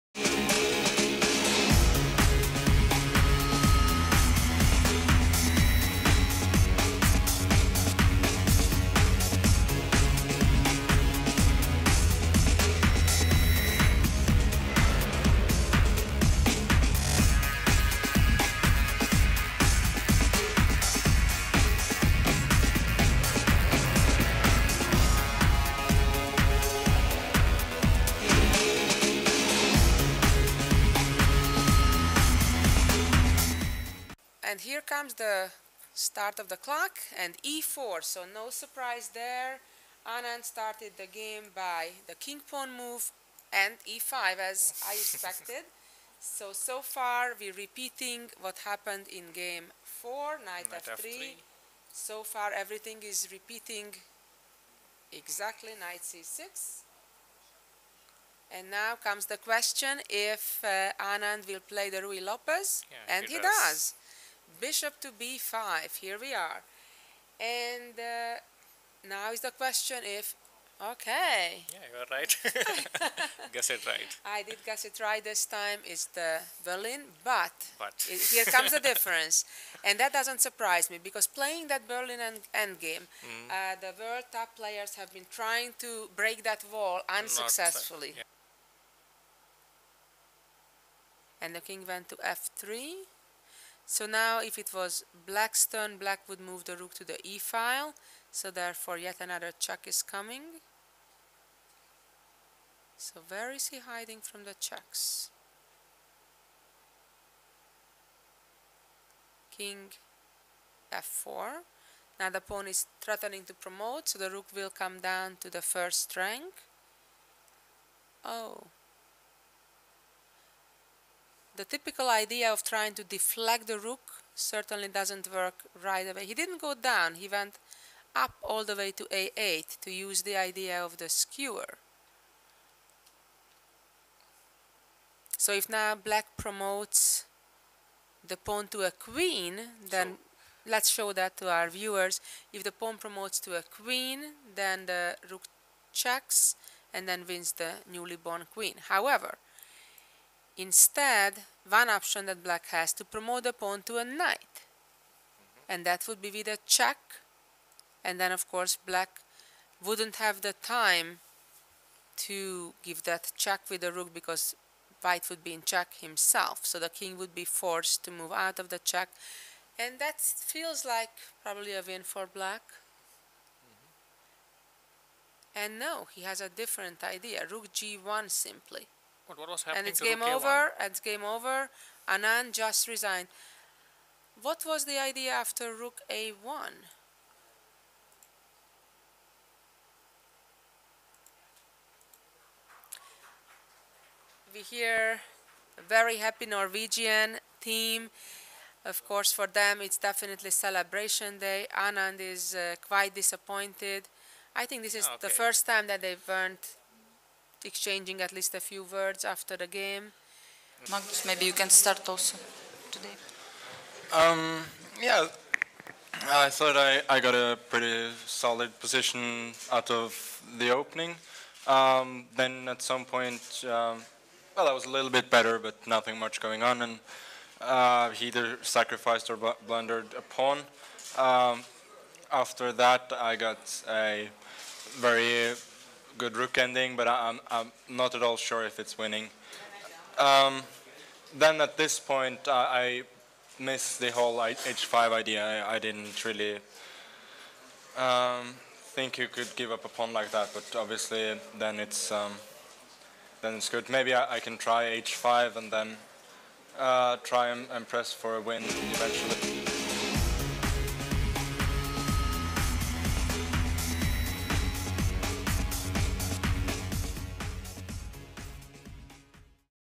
FWCM 2013 Video Report Game 6
Magnus Carlsen scored a second consecutive victory in the FIDE World Championship in Chennai to increase the lead to 4-2 after six games of play.
Sunday is the rest day, the second half of the match continues on Monday with Anand playing white pieces.